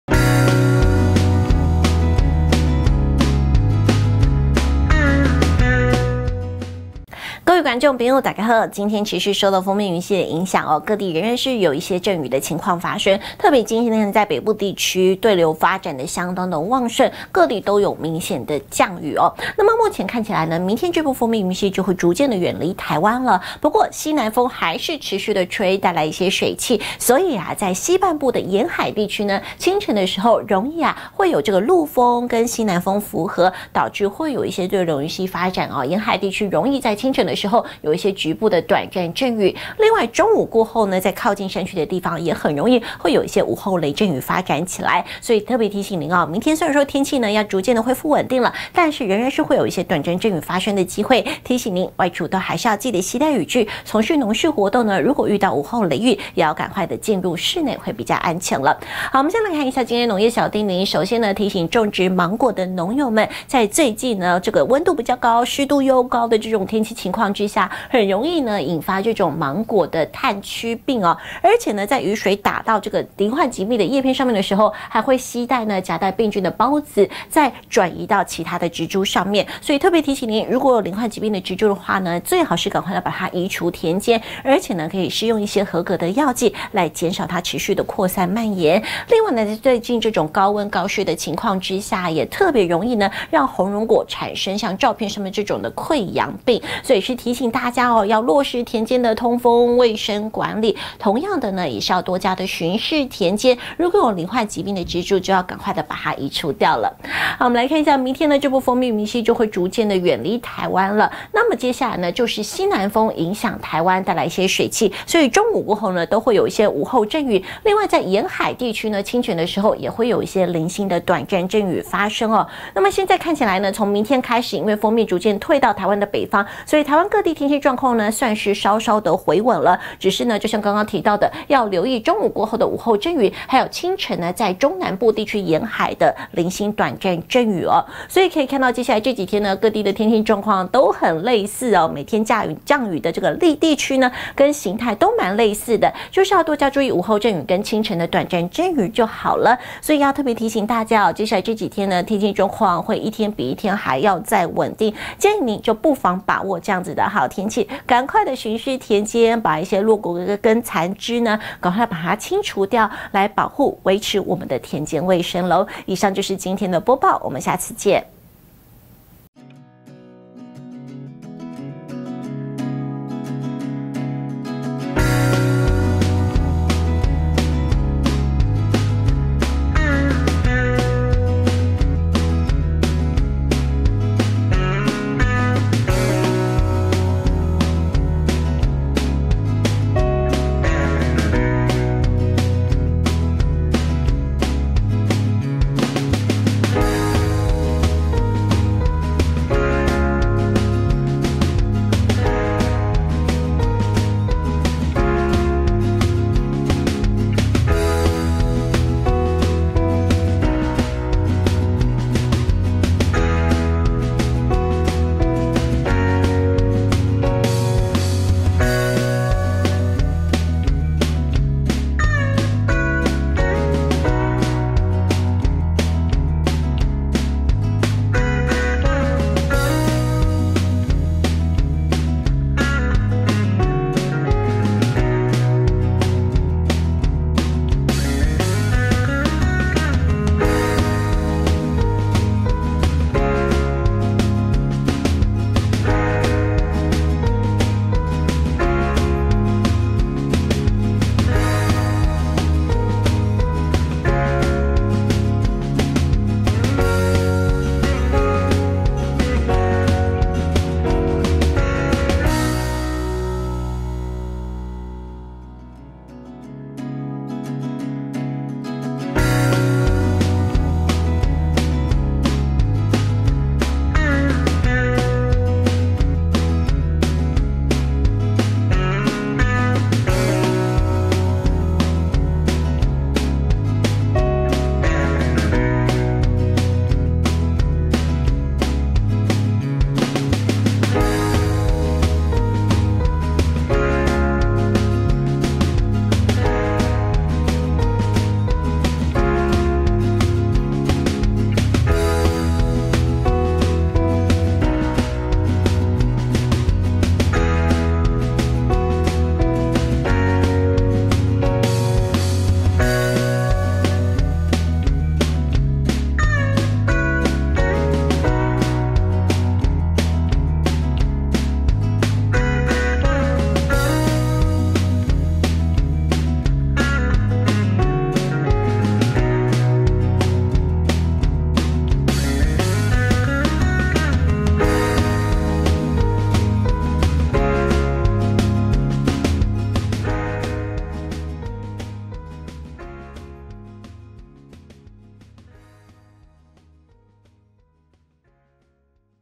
農業氣象 109/05/29 沿海清晨易有陣雨，山區午後亦有雷雨。
逐漸轉為西南風環境。
沿海清晨易有偶陣雨。
山區午後亦有雷陣雨。
入夜後天氣逐漸好轉。